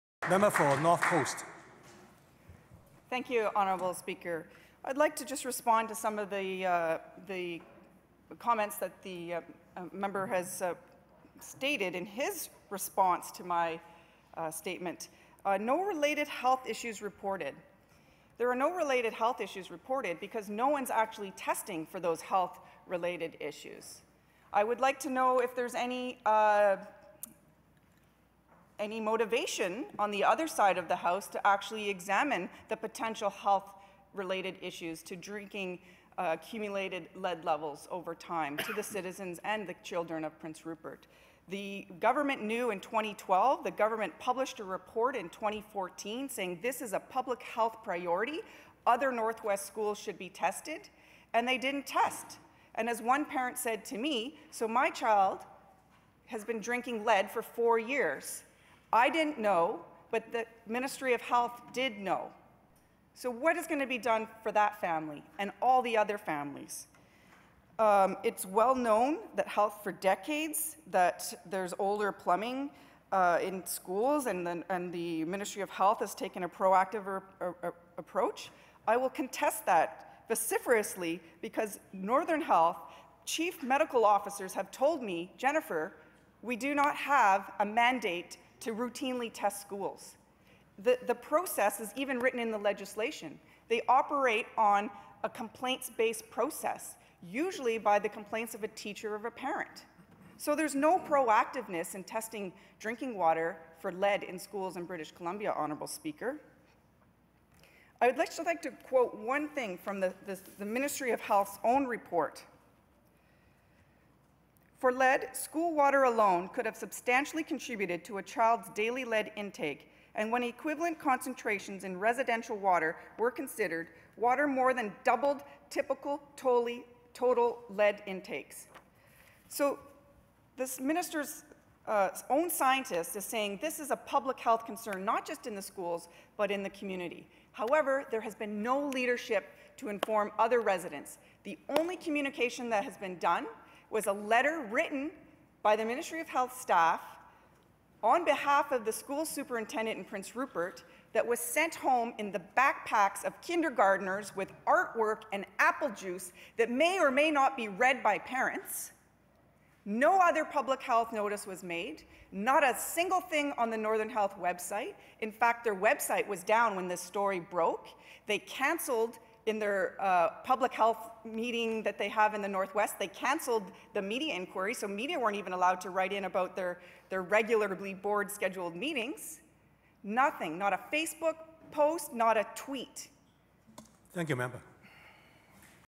MLA Rice demands regular water testing in schools
North Coast MLA Jennifer Rice is demanding routine testing of water in schools across the province after elevated lead levels were found in 3 Prince Rupert schools last month.